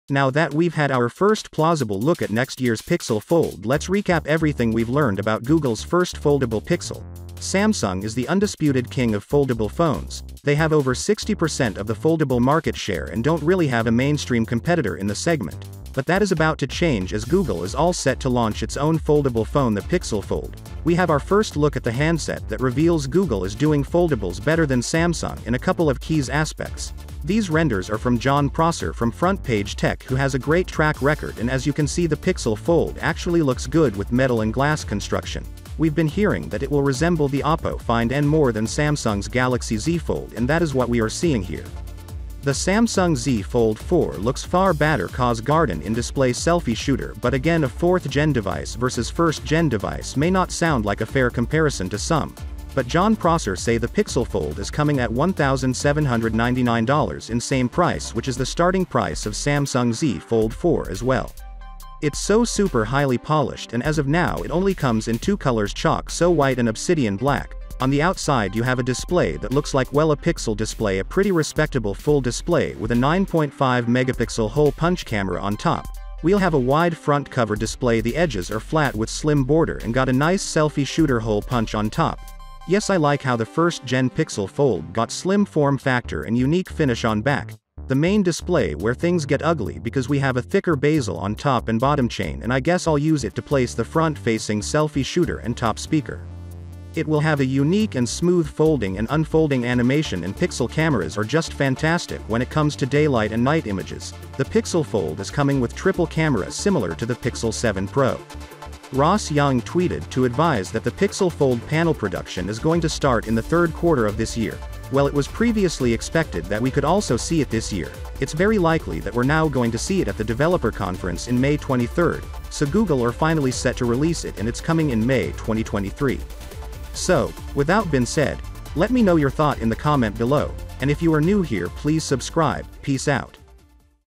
Google Pixel Fold - FRUITFUL SURPRISE!!
Now that we’ve had our first plausible look at next year’s pixel fold let’s recap everything we’ve learned about Google’s first foldable pixel, Samsung is the undisputed king of foldable phones, they have over 60 percent of the foldable market share and don’t really have a mainstream competitor in the segment, but that is about to change as Google is all set to launch its own foldable phone the pixel fold, we have our first look at the handset that reveals Google is doing foldables better than Samsung in a couple of keys aspects, these renders are from Jon prosser from front page tech who has a great track record and as you can see the pixel fold actually looks good with metal and glass construction, we’ve been hearing that it will resemble the oppo find N more than Samsung’s Galaxy z fold and that is what we are seeing here. The Samsung z fold 4 looks far batter cause garden in display selfie shooter but again a fourth gen device versus first gen device may not sound like a fair comparison to some, but John prosser say the pixel fold is coming at 1799 dollars in same price which is the starting price of Samsung z fold 4 as well. It’s so super highly polished and as of now it only comes in two colours chalk so white and obsidian black, on the outside you have a display that looks like well a pixel display a pretty respectable full display with a 9.5 megapixel hole punch camera on top, we’ll have a wide front cover display the edges are flat with slim border and got a nice selfie shooter hole punch on top, yes I like how the first gen pixel fold got slim form factor and unique finish on back, the main display where things get ugly because we have a thicker basil on top and bottom chain and I guess I’ll use it to place the front facing selfie shooter and top speaker. It will have a unique and smooth folding and unfolding animation and pixel cameras are just fantastic when it comes to daylight and night images, the pixel fold is coming with triple camera similar to the pixel 7 pro. Ross young tweeted to advise that the pixel fold panel production is going to start in the third quarter of this year, well it was previously expected that we could also see it this year, it’s very likely that we’re now going to see it at the developer conference in may 23, so Google are finally set to release it and it’s coming in may 2023.